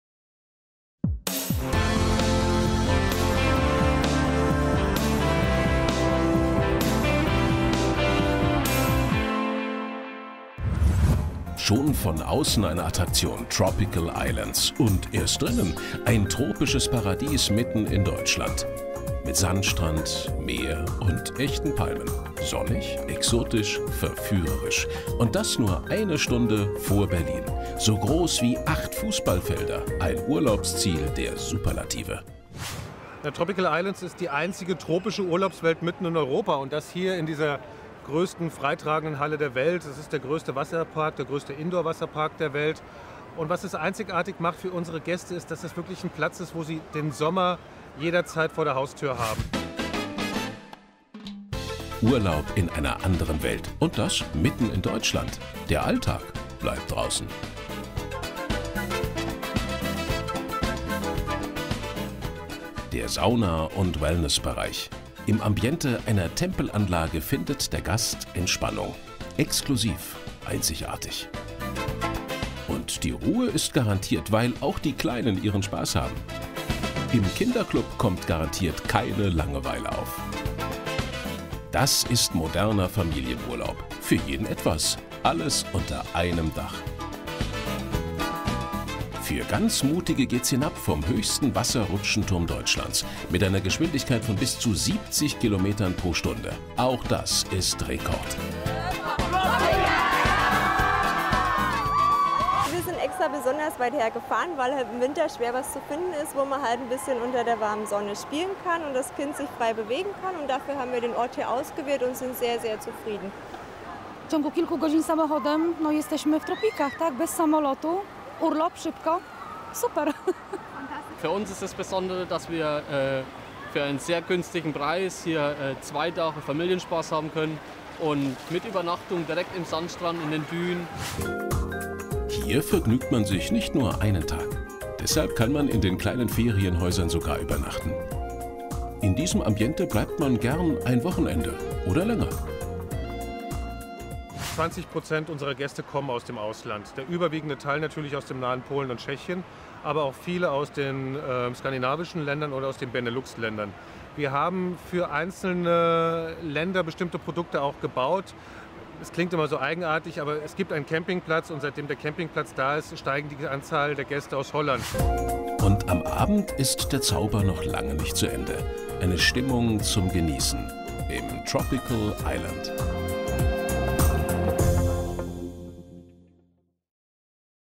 Sieger MarketingAward 2011 des OSV - „Tropical Islands Management GmbH"
Die „Tropical Islands Management GmbH" aus Krausnick (Landkreis Dahme-Spreewald ) ist Landessieger Brandenburg beim Tourismuswettbewerb des Ostdeutschen Sparkassenverbandes (OSV) MarketingAward „Leuchttürme der Tourismuswirtschaft" 2011. Europas größte tropische Urlaubswelt liegt eine knappe Autostunde südlich von Berlin im Landkreis Dahme-Spreewald. Seit 2004 bietet „Tropical Islands" seinen Besuchern auf einer Fläche, die etwa acht Fußballfeldern entspricht, Bade-, Wellness- und Freizeitattraktionen unter einem Dach. Die Raumtemperatur in dieser Indooranlage liegt ganzjährig konstant bei 26 Grad Celsius. Das Unternehmen verfolgt die Vision, sich zusammen mit dem Spreewald als europaweites Reiseziel zu positionieren. Gegenwärtig liegt der Anteil der Gäste aus ganz Europa bereits bei 20 Prozent. Beeindruckend sind die Konsequenz, der Ideenreichtum und die Professionalität bei der Erschließung internationaler Märkte. Die Basis hierfür ist ein Marketingkonzept. Besonders intensiv werden die polnischen und tschechischen Märkte bearbeitet. Vor Ort legen die Servicekräfte Wert auf Kundennähe und gehen nicht zuletzt durch mehrsprachige Informations- und Serviceleistungen auf die Bedürfnisse ihrer internationalen Gästen ein.